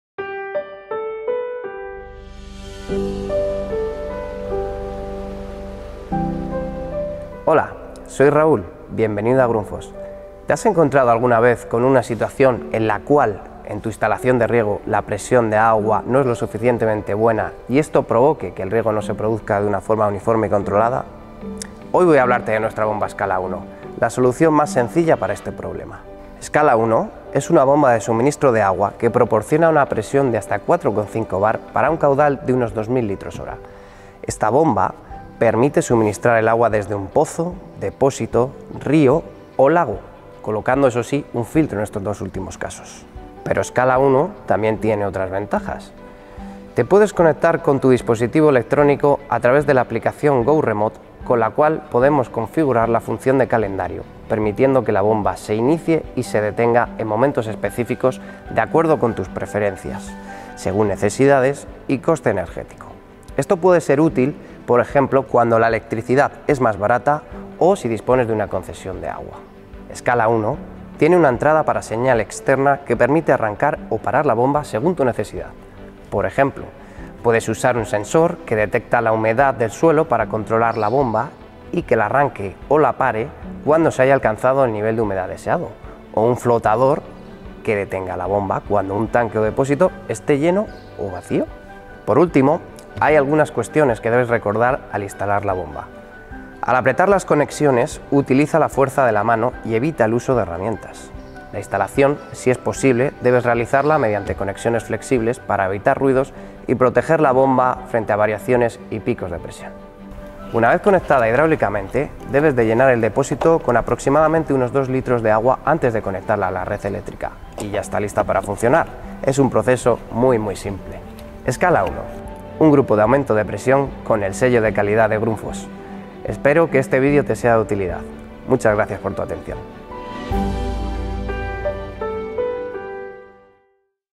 Cómo mejorar la presión de agua en un riego
Si tienes problemas de presión de agua en el riego, descubre cómo puedes solucionarlo de forma muy sencilla con nuestra bomba de aumento de presión de agua SCALA1 de Grundfos. La bomba cuenta con conexión bluettoth para poder controlarla desde un dispositivo inteligente con nuestra app Grundfos Go Remote. También dispone de función calendario para elegir los días y horas a las que la bomba debe funcionar en función de las necesidades y costes.